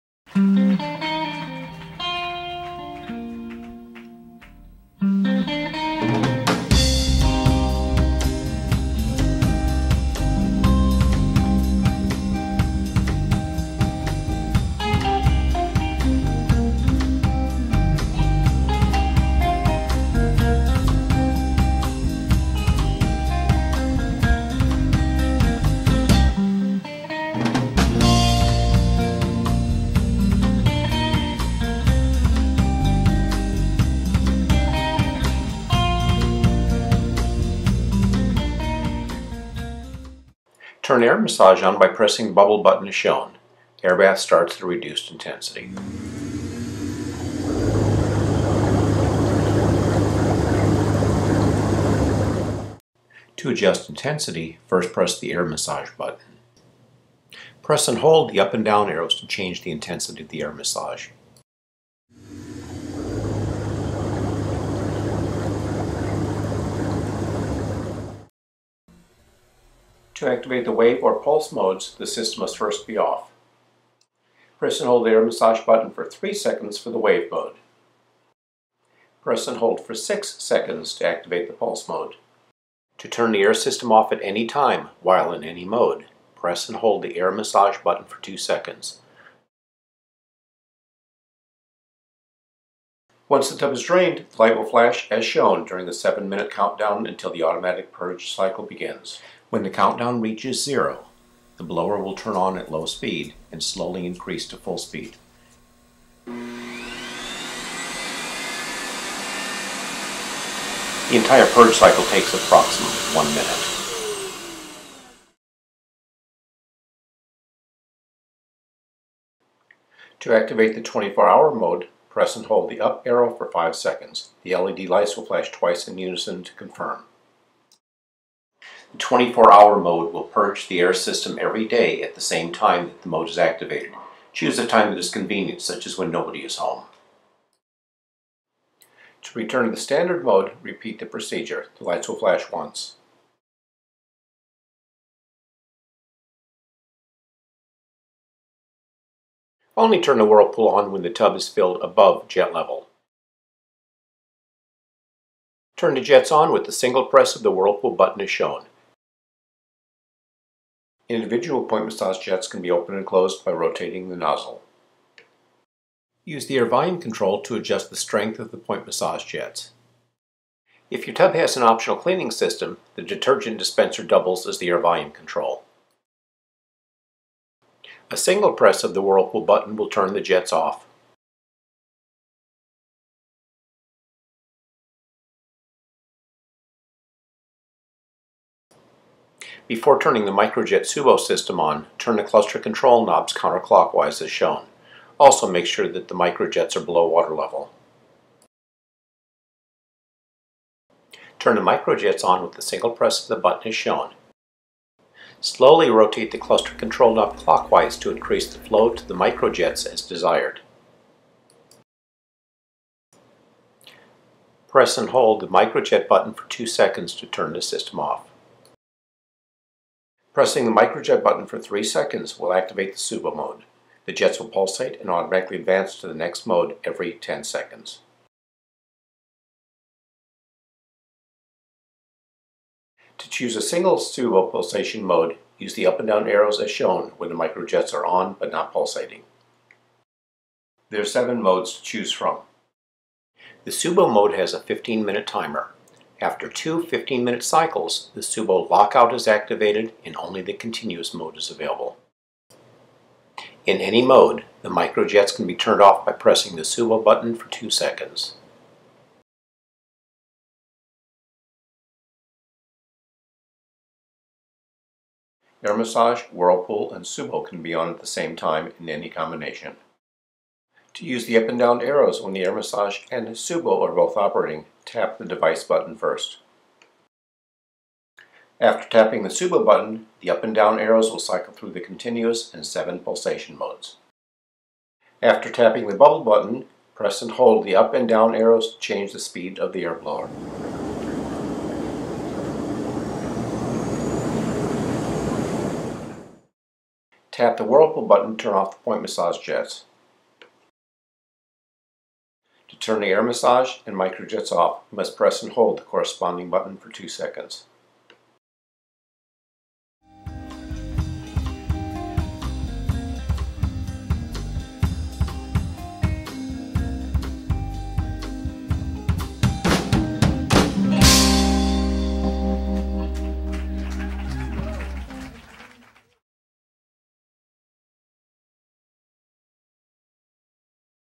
MTI Baths Keypad #8 Operation of Air Massage air bath / Ultra Therapy whirlpool
Easy to understand video showing how to operate the #8 keypad on MTI combination Air Massage air bath and Ultra Whirlpool tubs.